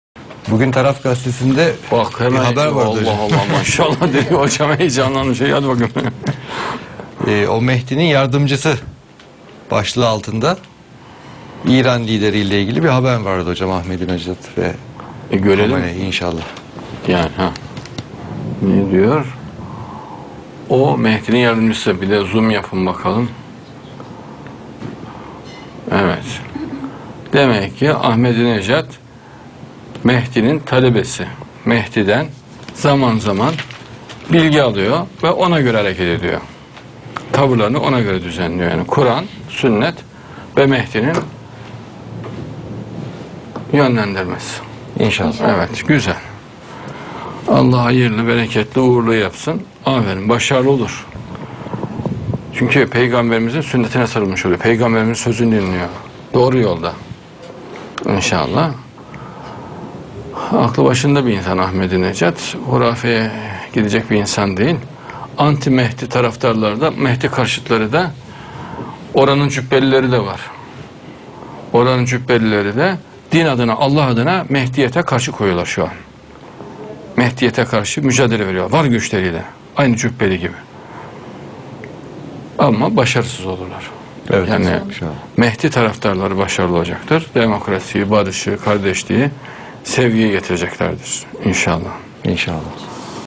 AHMEDI NEJAD HZ MEHDI'NIN TALEBESI,ONDAN BILGI ALIP ONA GÖRE HAREKET EDIYOR
AHMEDI NEJAD HZ MEHDI(AS)'NIN TALEBESI,ONDAN BILGI ALIP ONA GÖRE HAREKET EDIYOR

Sayin Adnan Oktar´in bütün kitaplarini adresinden temin edebilirsiniz.(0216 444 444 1)